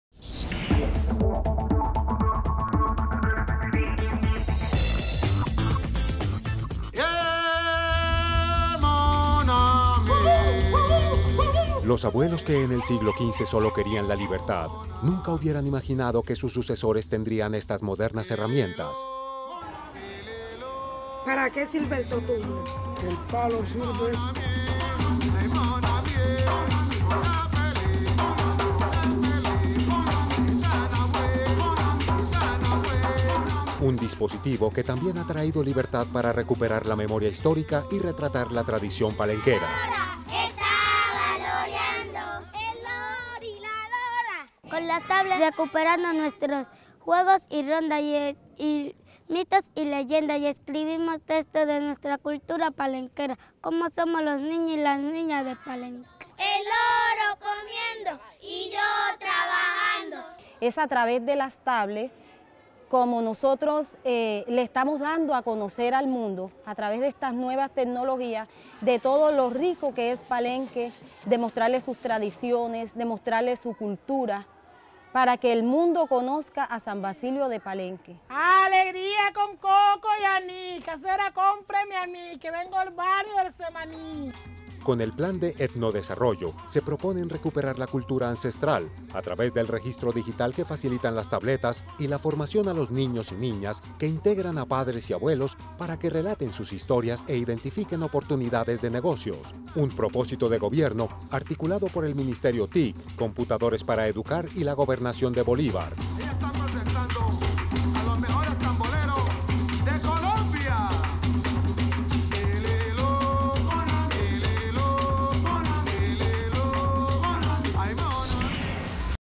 La cultura palenquera se inmortaliza gracias a las TIC
Con las tabletas la comunidad de Palenque está recuperando e inmortalizando su Cultura y tradiciones.